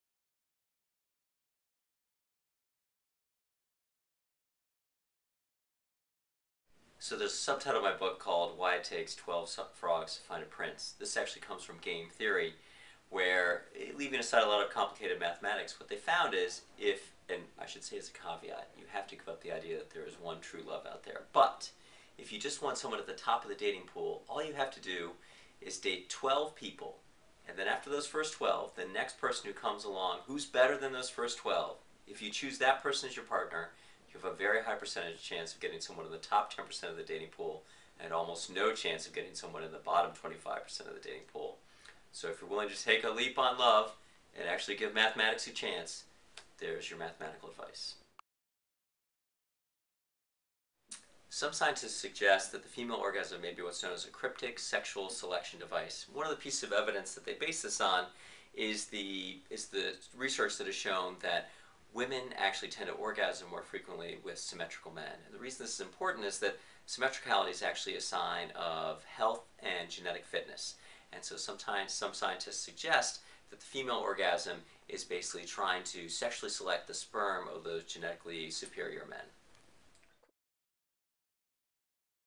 After the Show - Decoding Love
Andrew Trees explains how finding love is scientific